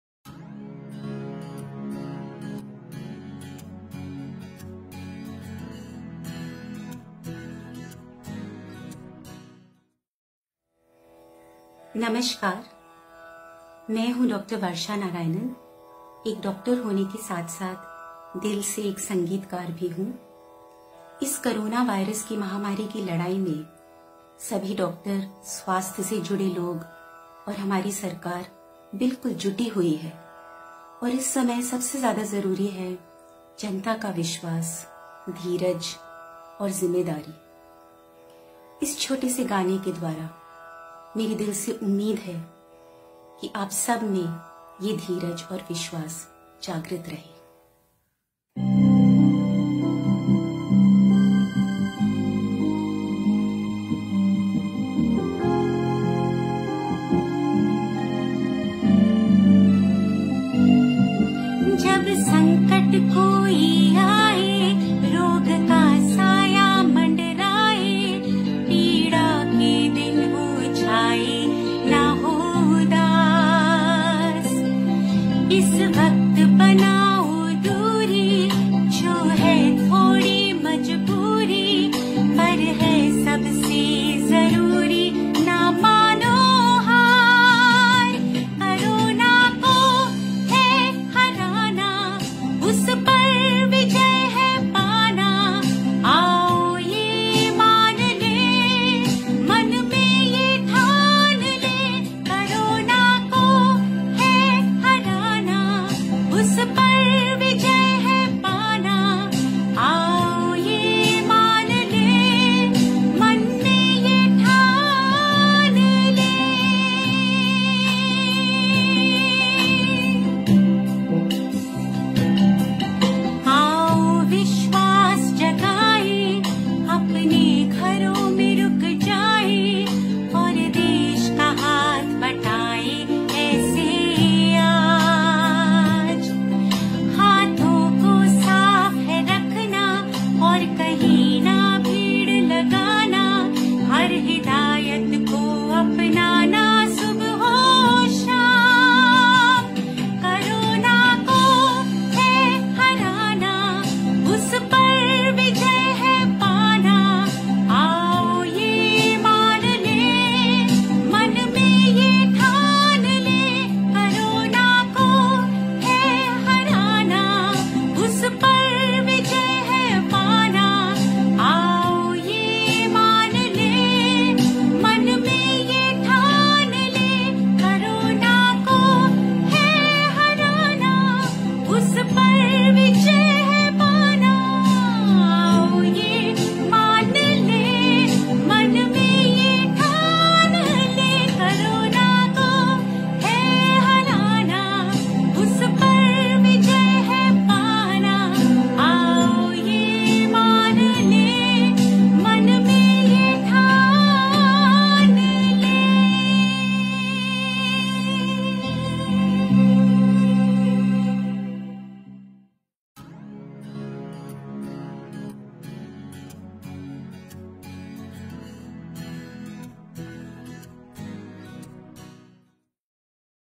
Musical Message Popular Musician And Doctor, Dr Varsha Narayanan | Corona Song
In a brief musical message, popular musician and doctor, Dr. Varsha Narayanan applaud the efforts of all healthcare, essential services workers and the Government in fighting the Pandemic. Watch to know more!